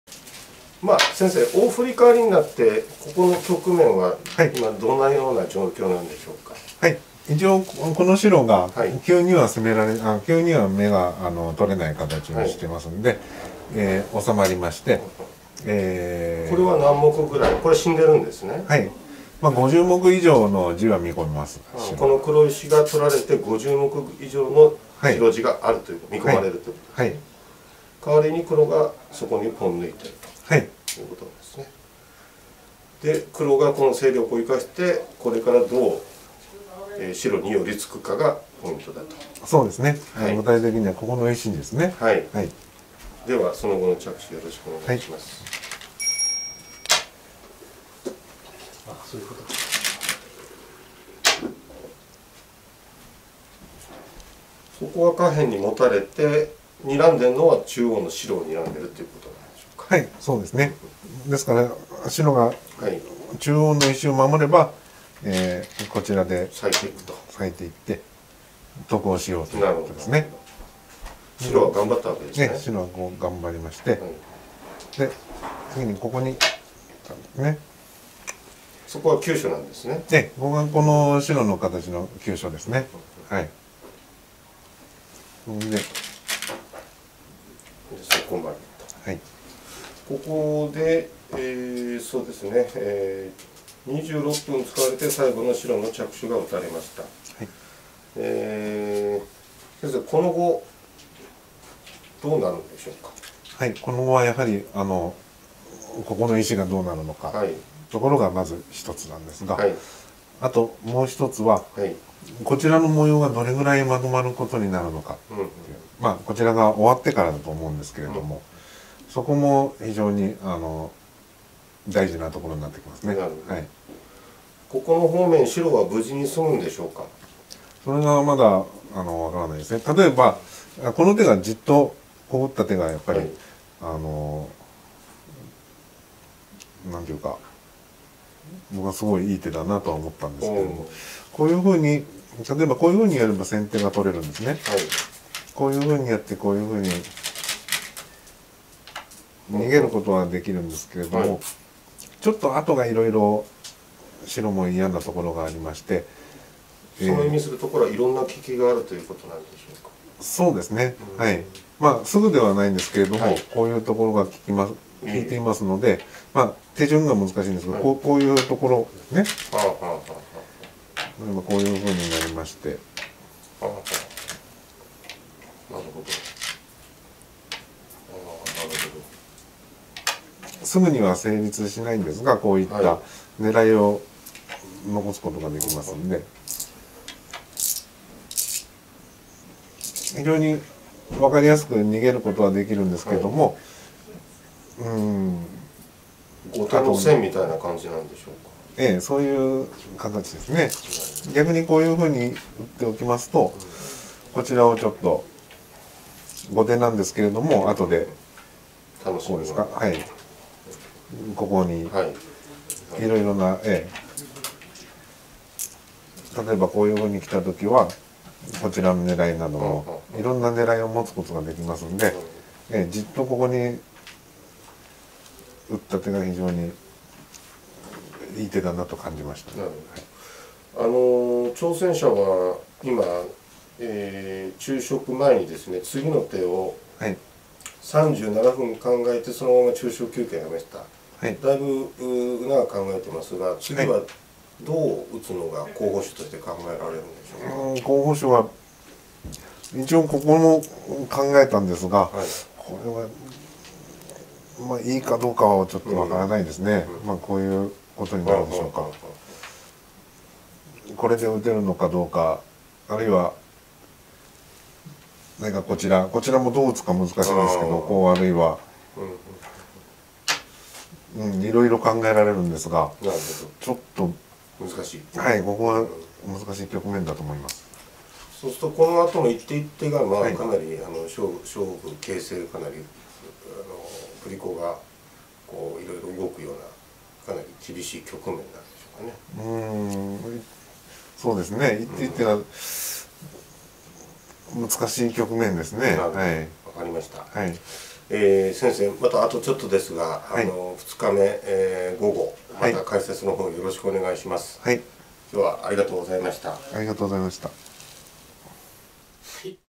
【第42期囲碁名人戦七番勝負・第5局】秋山次郎九段の局面解説 2日目昼食休憩
新聞解説の秋山次郎九段に2日目の昼食休憩局面について伺いました。